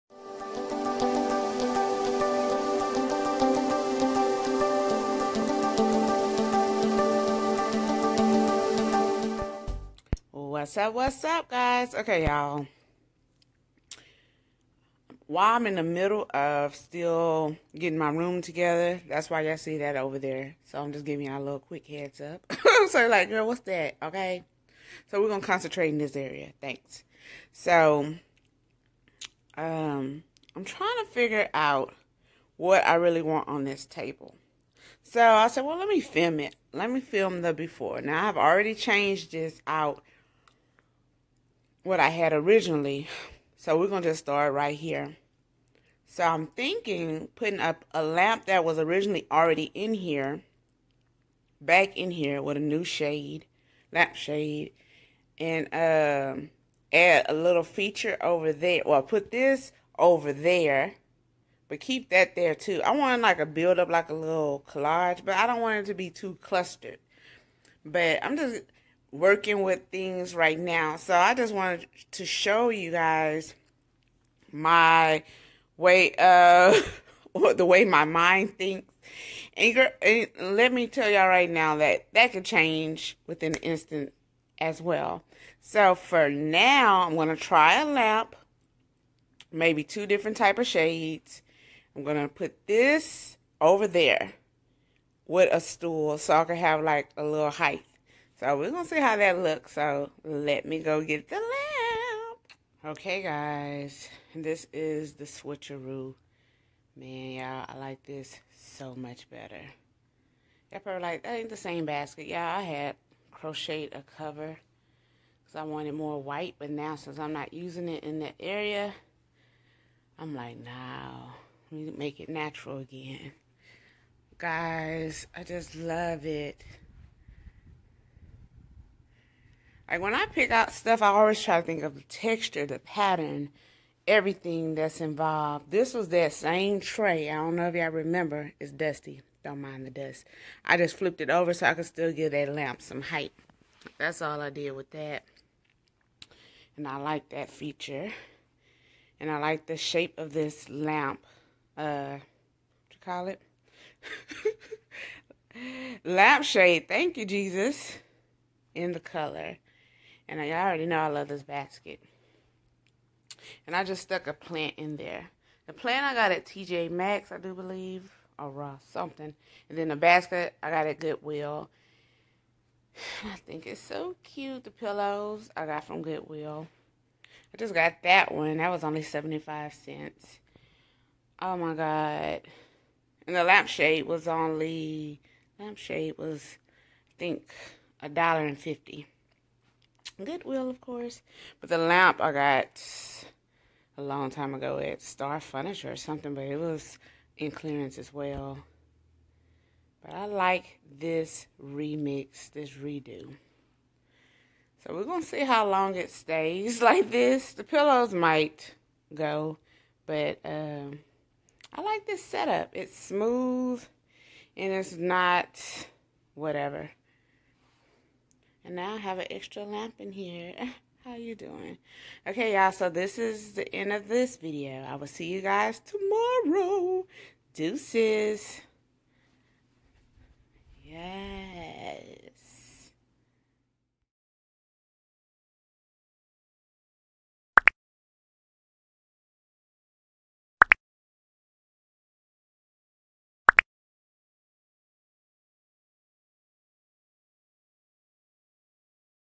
Home Decor Living Room Furniture Remix Vlogging 2021
Hello Y'all!  From room makeovers to losing weight I will try to complete my goals before my birthday on May 1!  So join in the fun and watch my vlog this month.

Furniture Remix
Vlogging 2021